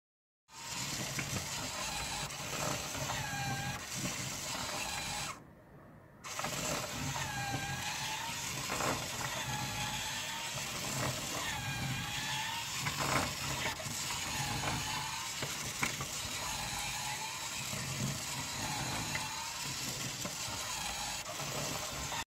Fusion360, 큐비콘 싱글플러스로 만든 메커니컬 2족보행장치 모형 "두바리" 3.0입니다 [이족보행장치]
[도서] 레고로 배우는 기계의 운동원리 152, Fusion 360, 3D프린터(큐비콘 싱글플러스), 아두이노, DC 모터를 사용해서 만든 모형 "두바리 3.0"의 작동영상입니다